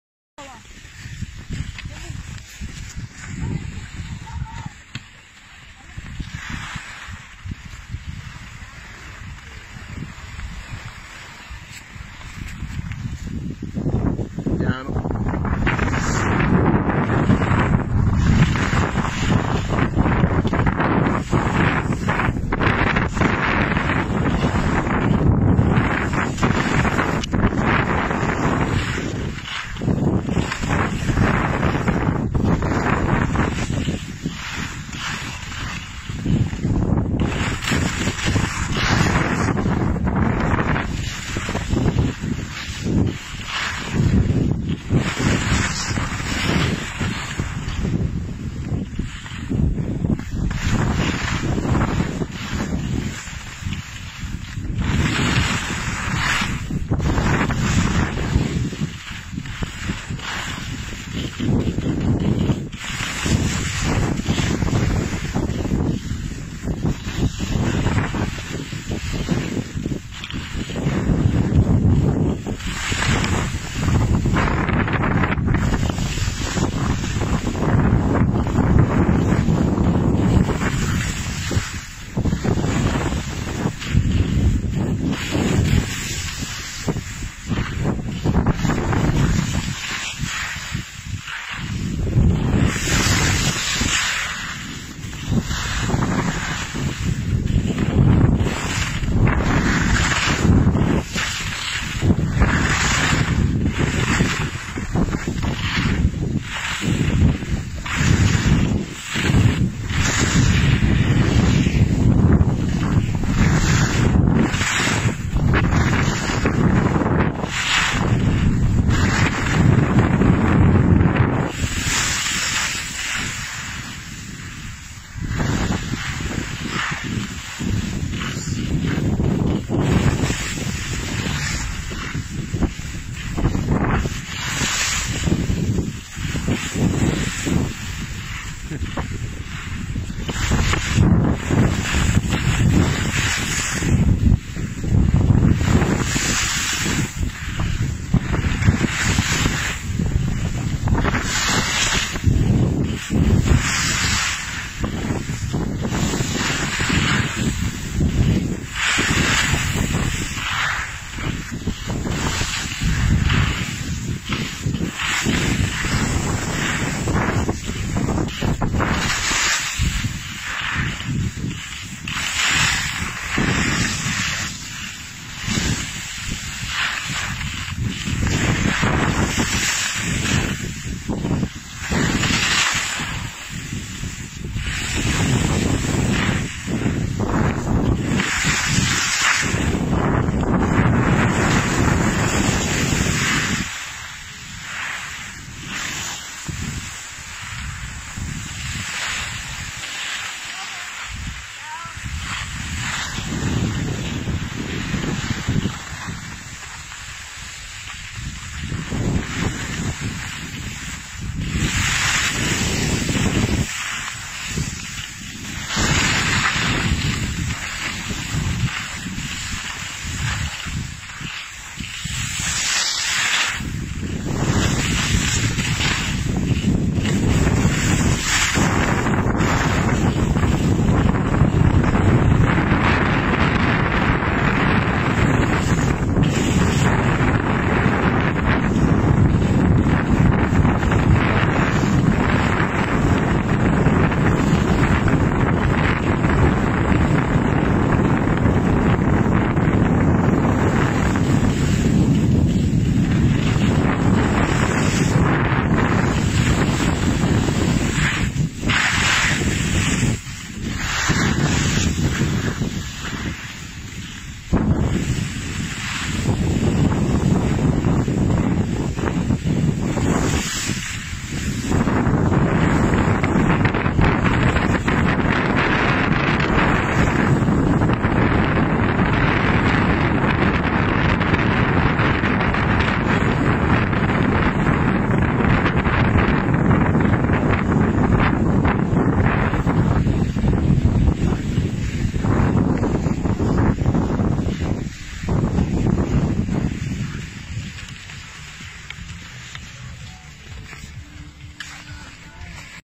Pista nera del Corno d'Aola a Ponte di Legno
Ed ora il video della Pista nera del Corno d'Aola a Ponte di Legno. Una nera molto bella ed impegnativa che mette alla prova il vostro livello tecnico.